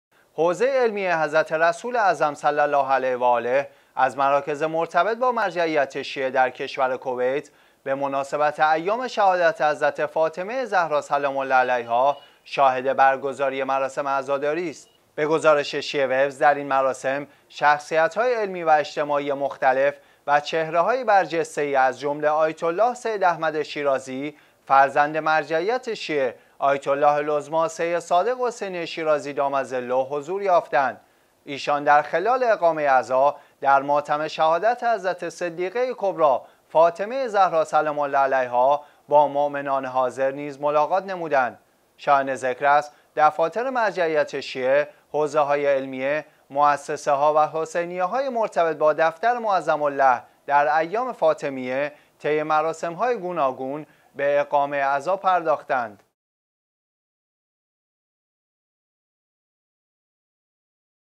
عزای فاطمی در حوزه علمیه حضرت رسول اعظم صلی الله علیه وآله در کشور کویت
حوزه علمیه حضرت رسول اعظم صلی الله علیه وآله، از مراکز مرتبط با مرجعیت شیعه در کشور کویت به مناسبت ایام  شهادت حضرت فاطمه زهرا سلام الله علیها شاهد برگزاری مراسم عزاداری است.
به گزارش شیعه ویوز، در این مراسم شخصیت های علمی و اجتماعی مختلف و چهره های برجسته ای از جمله آیت الله سید احمد شیرازی، فرزند مرجعیت شیعه آیت الله العظمی سید صادق حسینی شیرازی دام ظله حضور یافتند.
ایشان در خلال اقامه عزا در ماتم شهادت حضرت صدیقه کبری فاطمه زهرا سلام الله علیها با مؤمنان حاضر نیز ملاقات نمود.
شایان ذکر است، دفاتر مرجعیت شیعه، حوزه های علمیه، موسسه ها و حسینیه های مرتبط با دفتر معظم له در ایام فاطمیه طی مراسم های گوناگون به اقامه عزا پرداخته اند.